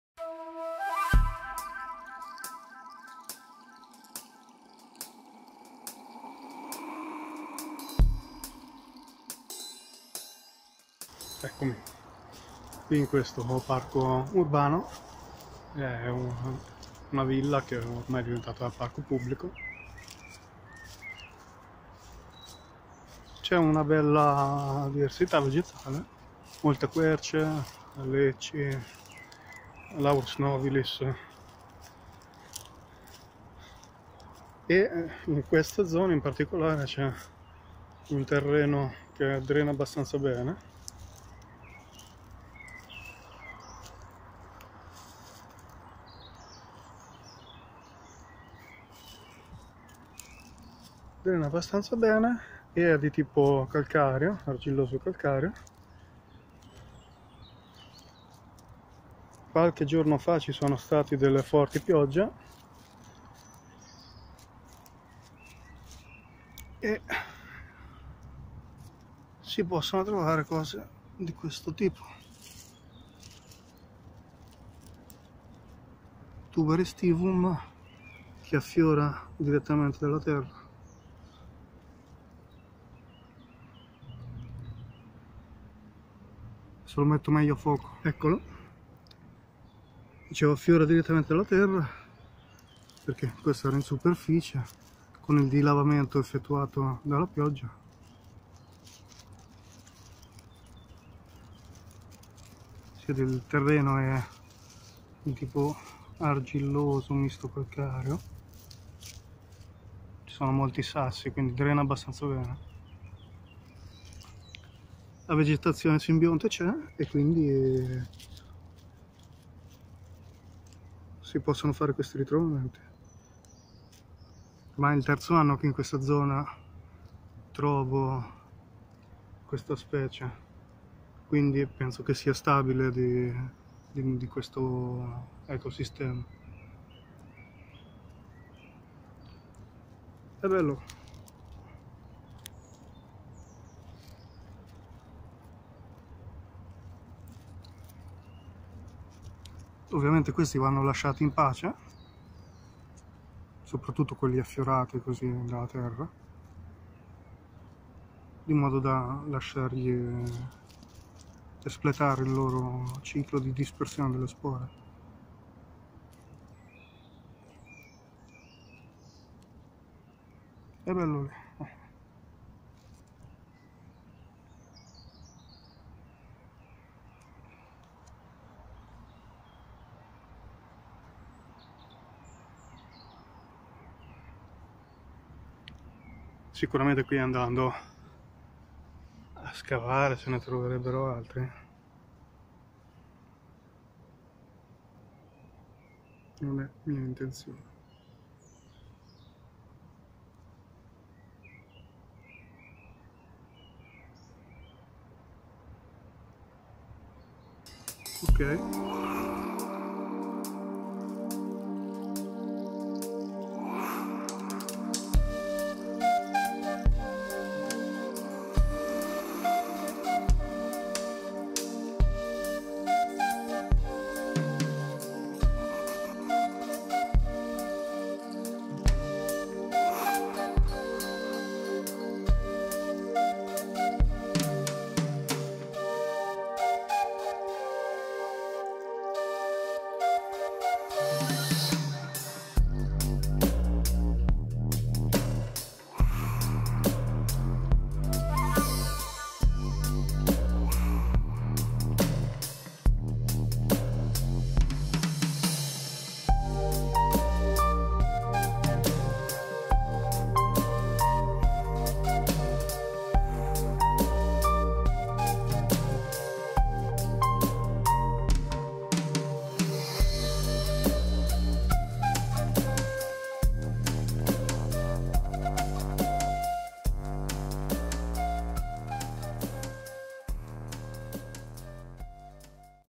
Tuber aestivum , il tartufo scorzone, un ascomicete ipogeo
Ritrovamento di un esemplare di Tuber aestivum Vittadini in un parco urbano  ricco di varie specie di Quercie. Il Tuber aestivum è un ascomicete che stringe simbiosi micorriziche con molti alberi, spesso dopo forti pioggie è facile ritrovare esemplari parzialmente affiorati dal terreno. Nel video una tabella con le principali specie arboree che stringono simbiosi con questo fungo ed alcune foto che ho scattato alle spore. Ritrovamento Giugno 2020